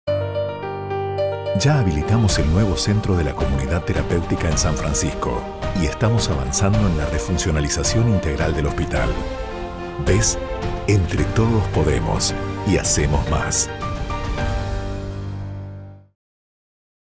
Hospital San Francisco
Habilitamos el nuevo centro de la comunidad terapéutica de San Francisco y refuncionalizamos el hospital ¿Ves? Entre todos podemos y hacemos más